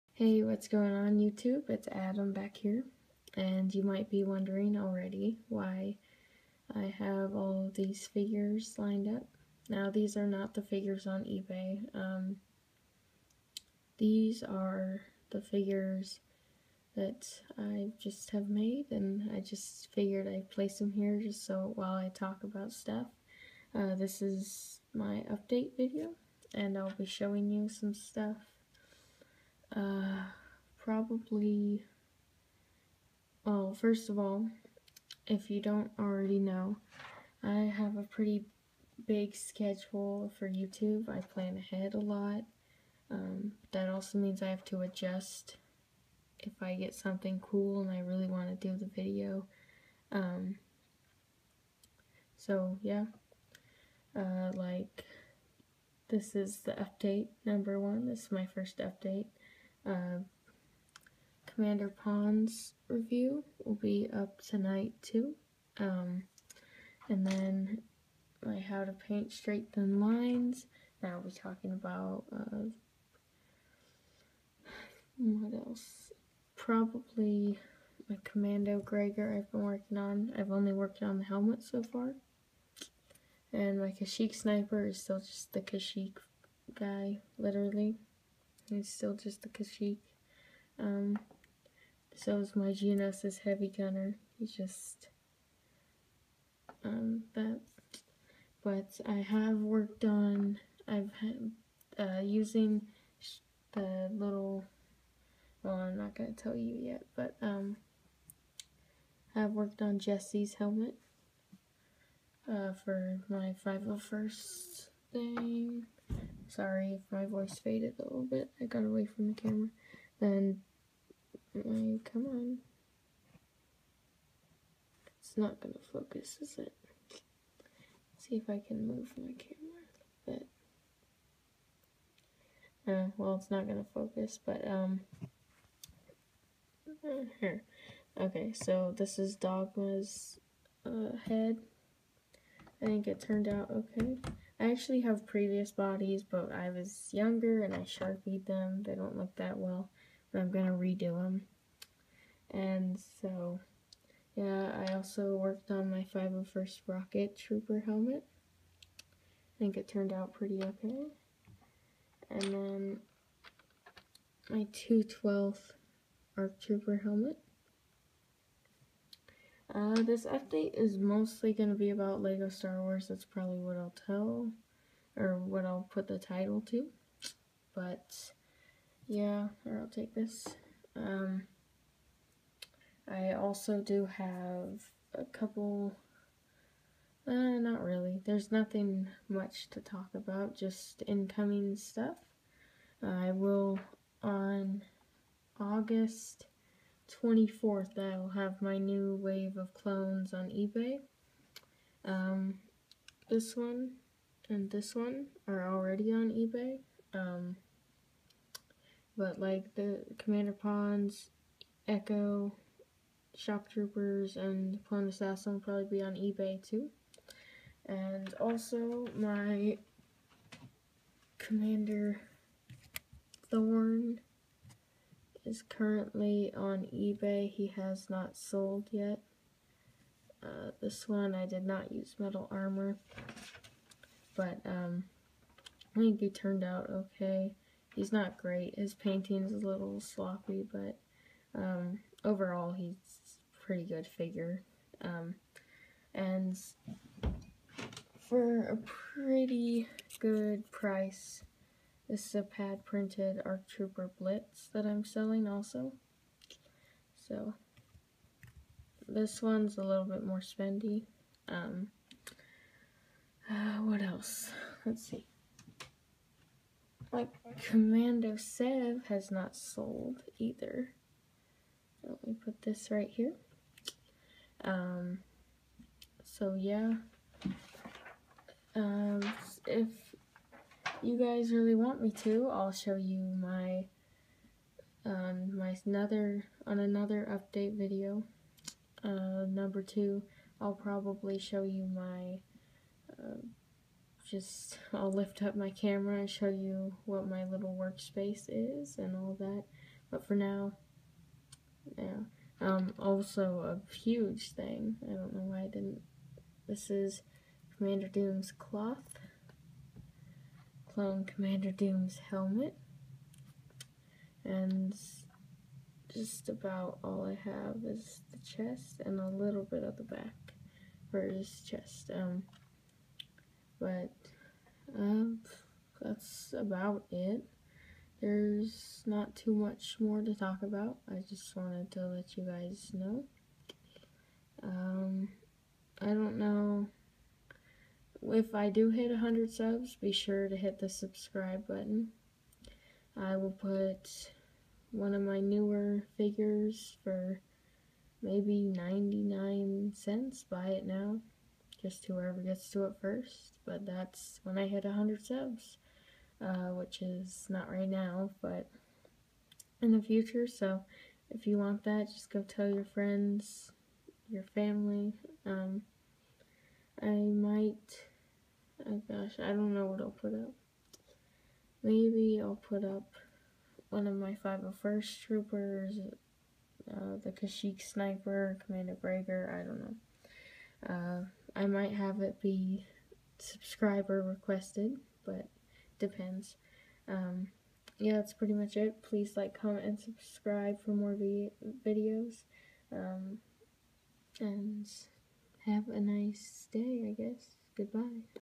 Lego Update #1 ebay, and upcoming stuff
This is my first update video and just talking about my custom and eBay figures